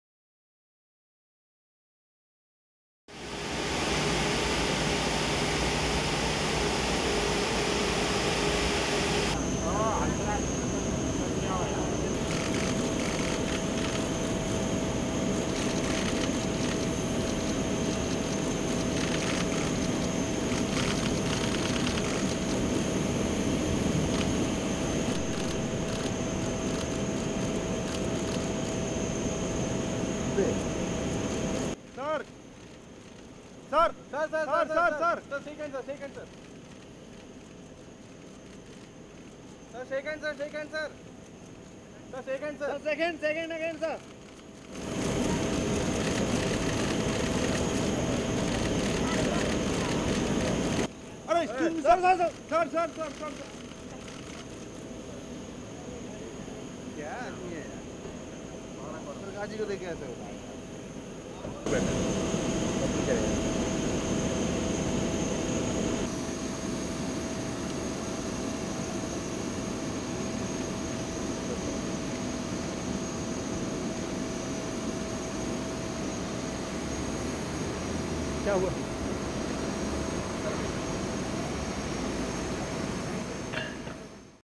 30 Oct, 2017 - Italian PM Gentilioni arrives in India on two-day visit
30 Oct, 2017 (ANI): Italian PM Gentilioni arrives in India on two-day visit